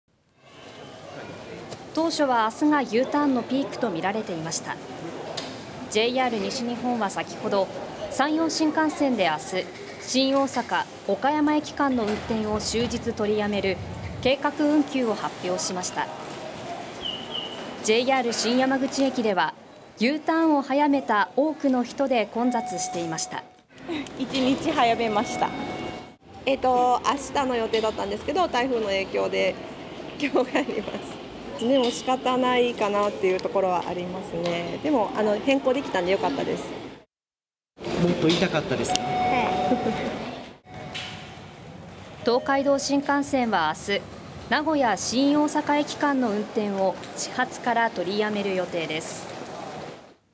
台風７号の影響でＵターン前倒し…ＪＲ新山口駅では混雑も
台風7号は、盆のUターンラッシュを直撃する予想です。

新幹線の計画運休を受けて山口県のJR新山口駅では、Uターンを前倒しする人で混雑しています。

当初は15日がUターンのピークとみられていました。
JR西日本は山陽新幹線で15日、新大阪・岡山駅間の運転を、終日取りやめる計画運休を発表しました。
JR新山口駅では、Uターンを早めた多くの人で混雑していました。

埼玉へ戻る利用者は
「（Uターンを）1日早めました」

親子は
「（Uターンは）あした（１５日）の予定だったんですけど、台風の影響できょう帰ります。しかたないかなというところはありますね。でも変更できたんでよかったです」
また、東海道新幹線は15日、名古屋・新大阪駅間の運転を始発から取りやめる予定です。